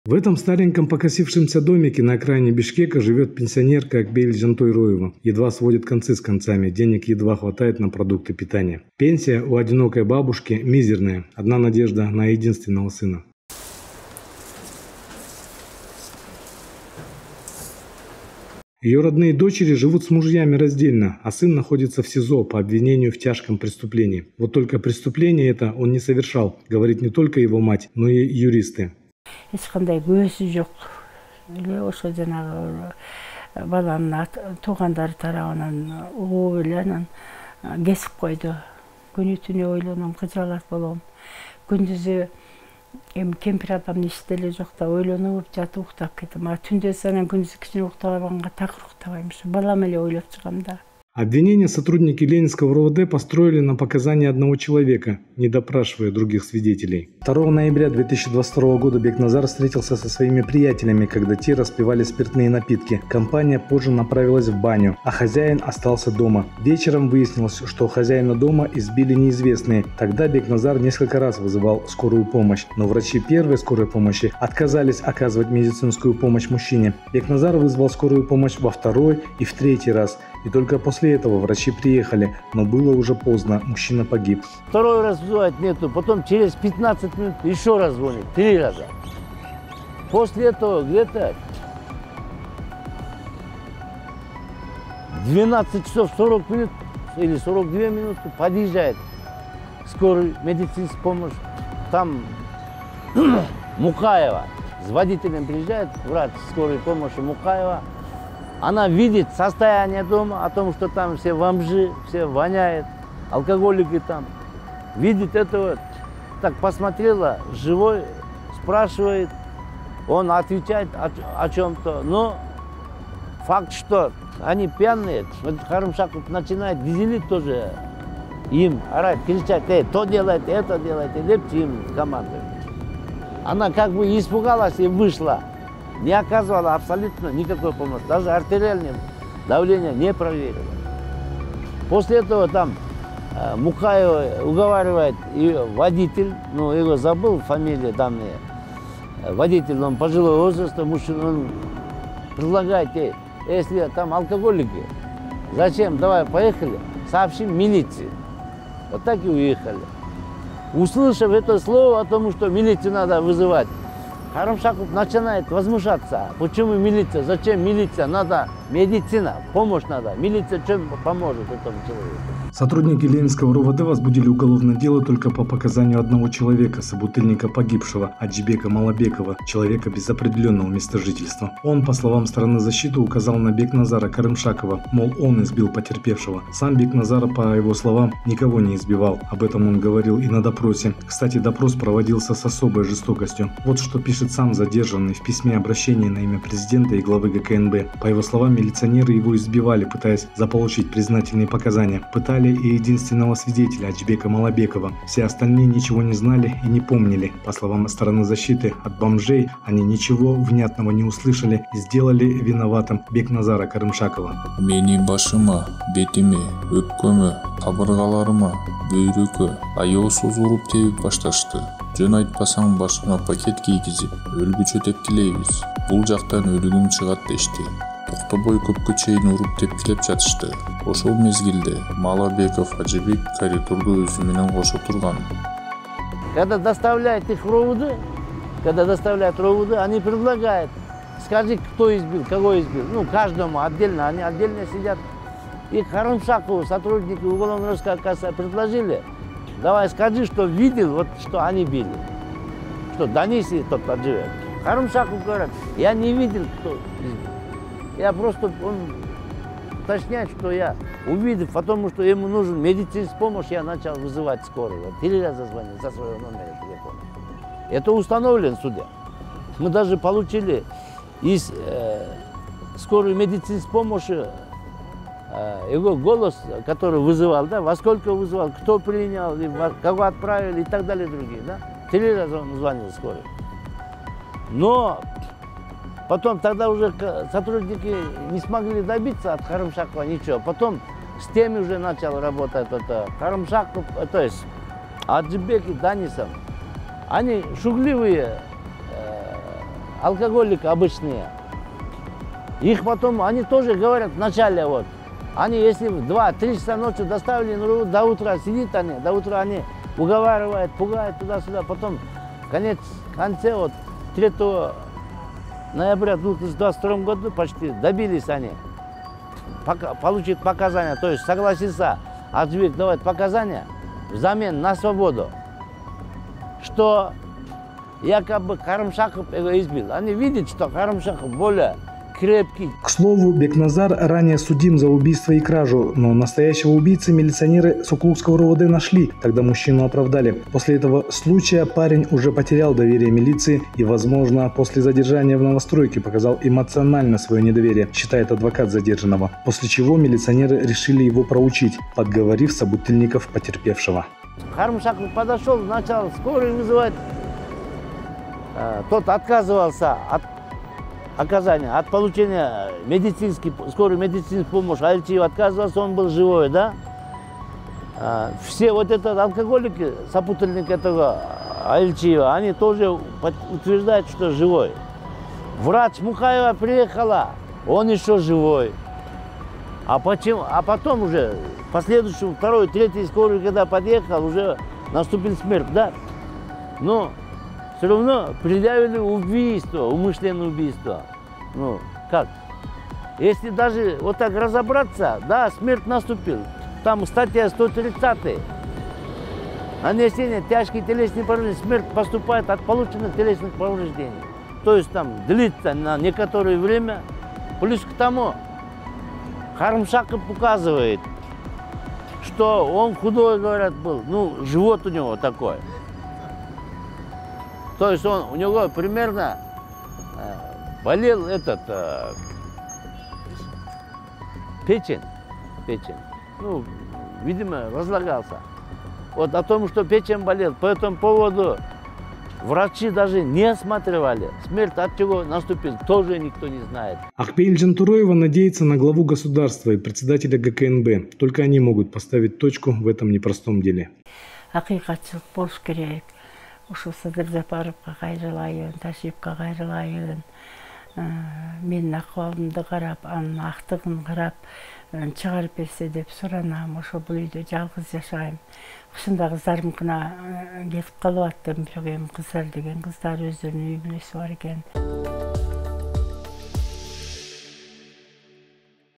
Акпейл Жантороева: "Баламды куноосуз камашты!"
В стареньком покосившемся домике на окраине Бишкека живёт Акпеил Жантороева. Едва сводит концы с концами. Денег едва хватает на продукты питания. Пенсии одинокой бабушке не хватает. Одна надежда на единственного сына. Её родные дочери живут с мужьями раздельно, а сын находиться в СИЗО по обвинению в тяжком преступлении. Вот только преступление он это не совершал, говорит не только его мать, но и юристы. Обвинение сотрудники Ленинского РОВД построили на показании одного человека, не допрашивая других свидетелей. Суды тоже расмотрели дело однобоко. Акпеил Жантороева надеется на Главу Государства и Председателя ГКНБ. Только они могут поставить точку в этом непростом деле.